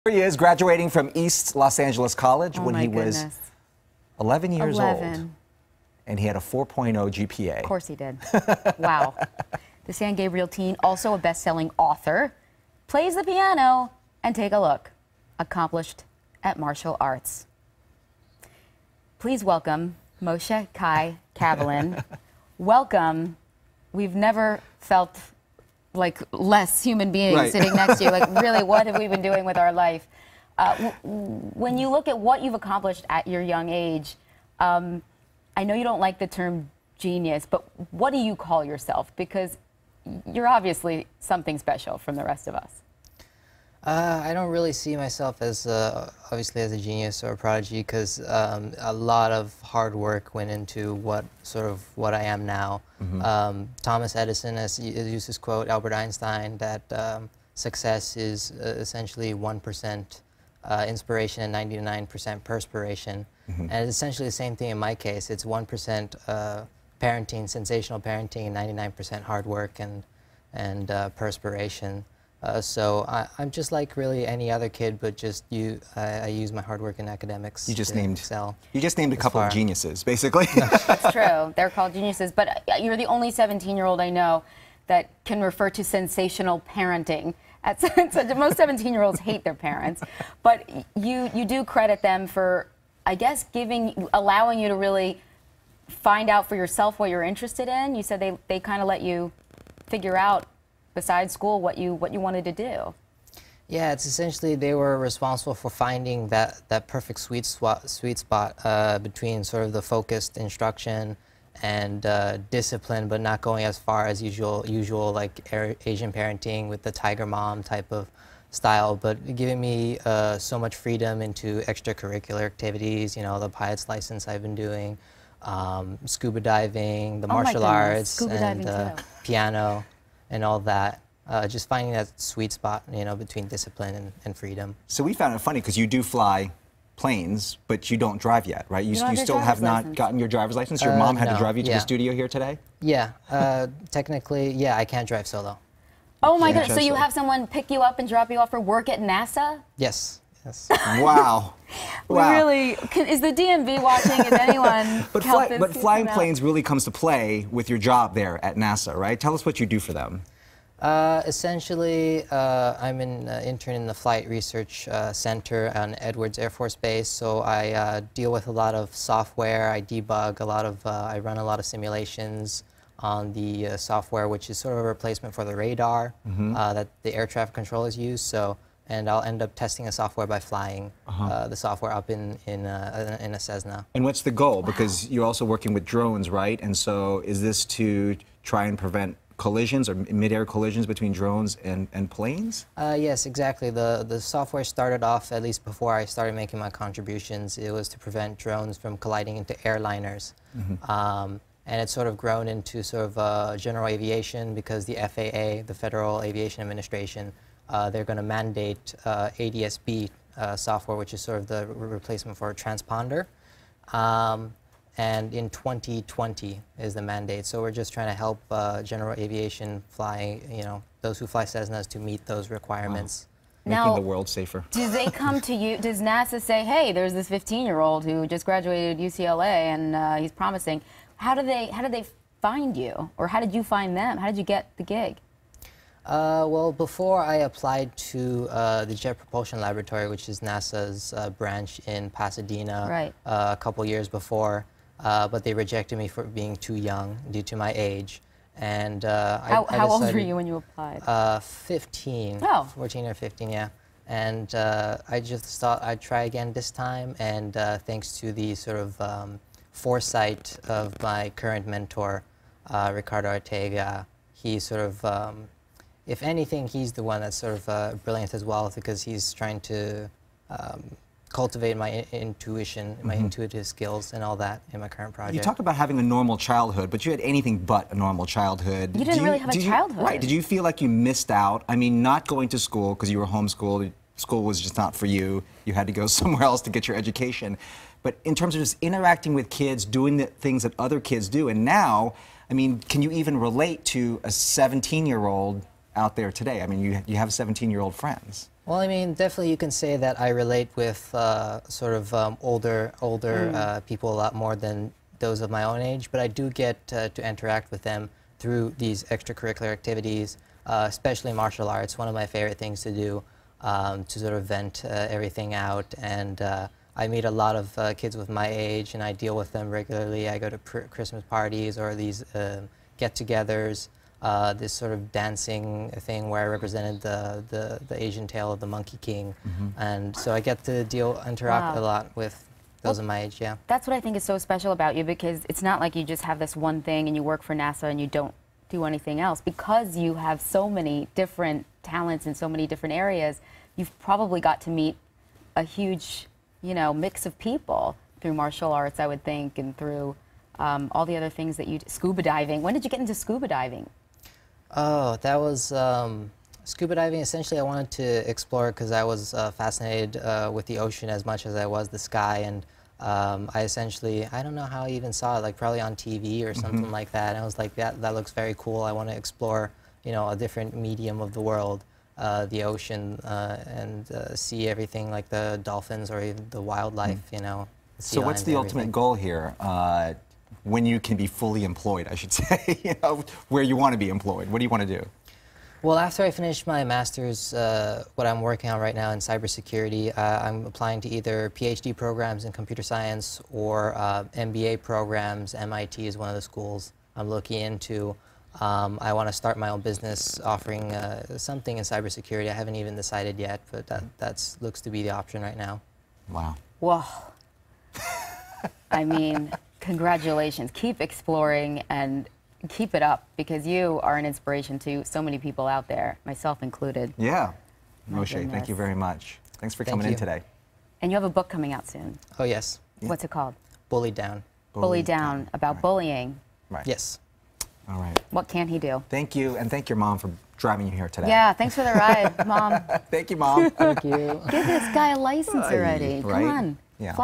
Teen graduates from UCLA at 15-years-old and now works for NASA
Moshe Cavalin is 17 and already works for NASA but wait until you hear what he accomplished by the time he was nine!